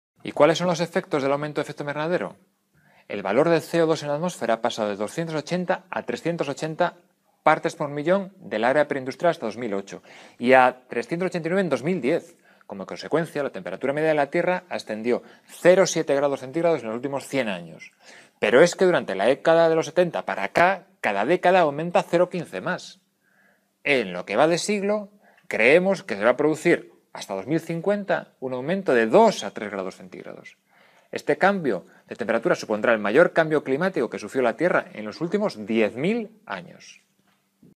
7.5 Consecuencias aumento efecto invernadero (I)
Consecuencias efecto invernadero Vídeos elaborado polos profesores de secundaria Xacobo de Toro e Jorge Gutiérrez durante a Licenza de Estudos outorgada pola Consellería de Educación, Xunta de Galicia, no curso 2014/15.